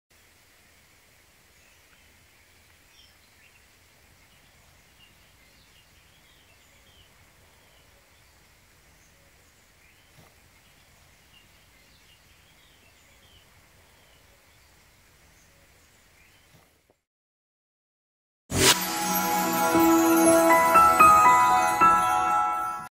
Walking Leaf just hatched this night. Phyllium Giganteum
in the night a walking leaf hatched ; it is still brown, hopefully it will turn green soon. great joy, the real Phyllium, a Phyllium Giganteum. original is 4k.